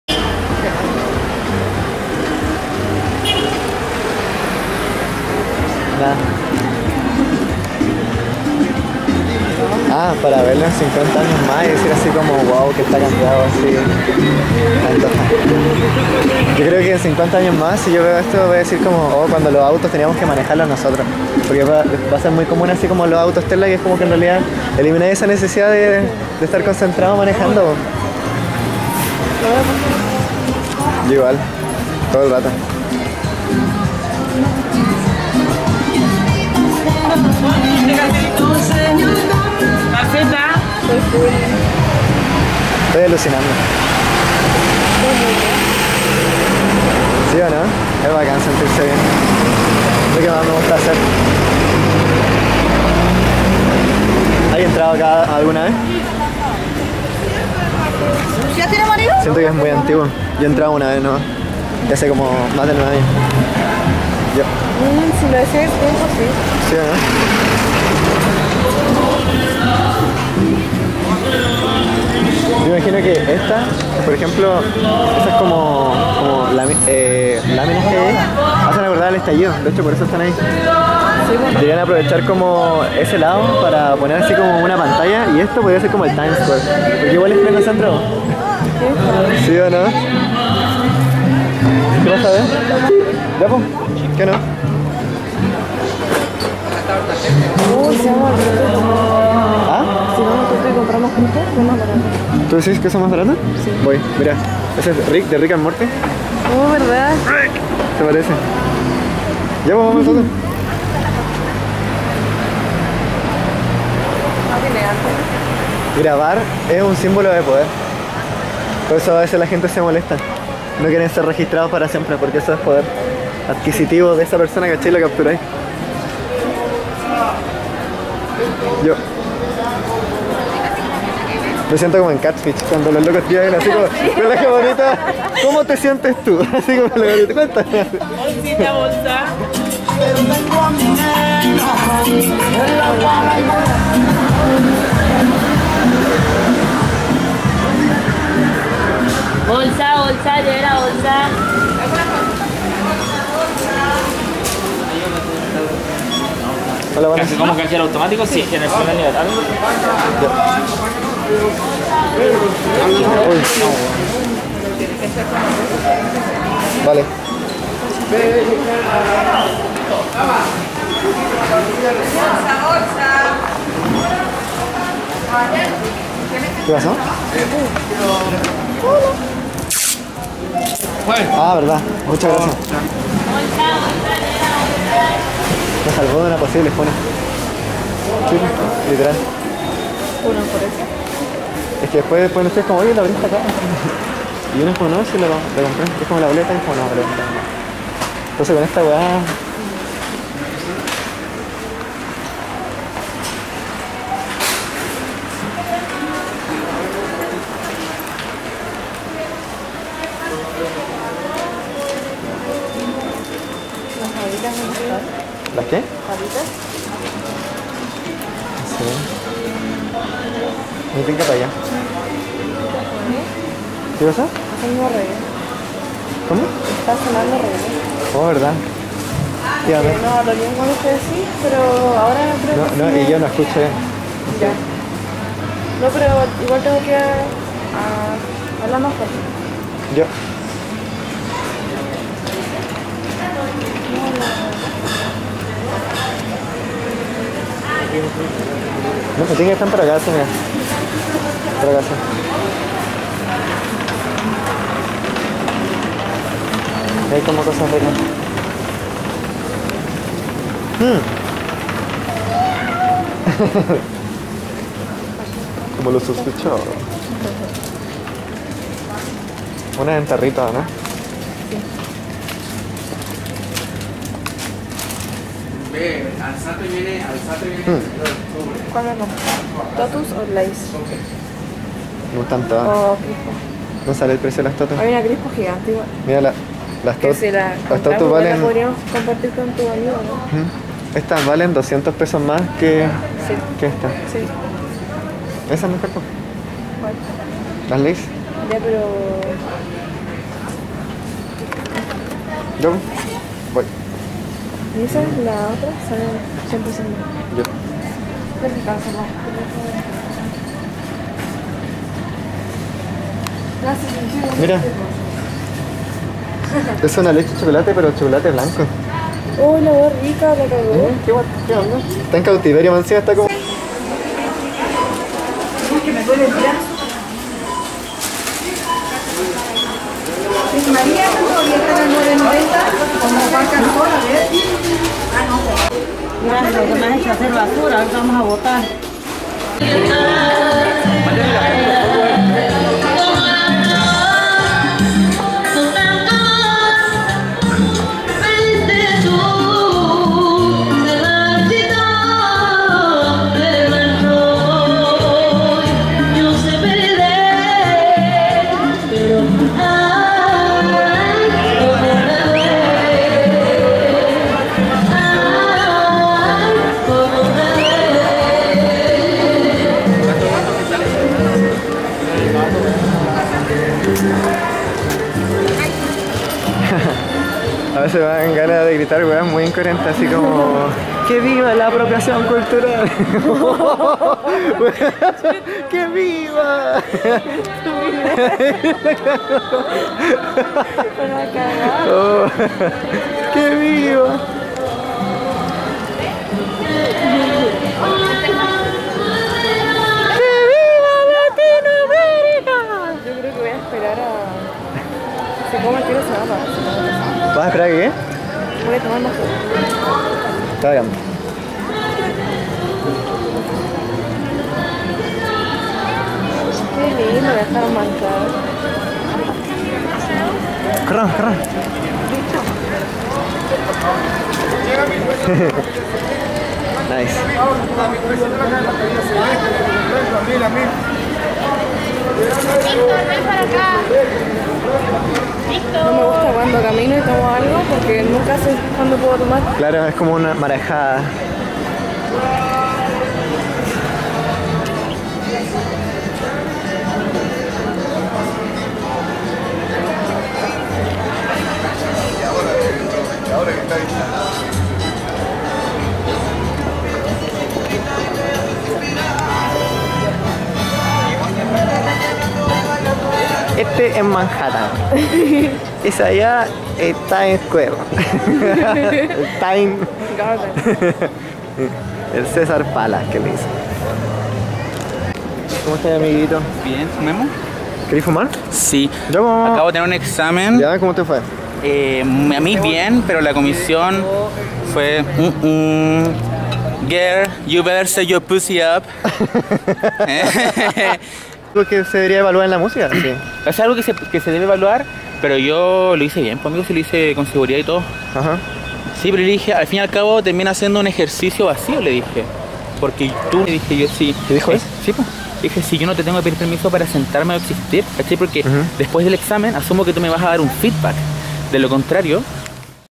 CALLES DE ANTOFAGASTA 30 JUN 2022
Esto es para verlo en 20 años más y decir ohh que está cambiado todo, cuando los autos se manejaban manualmente x nosotro mismos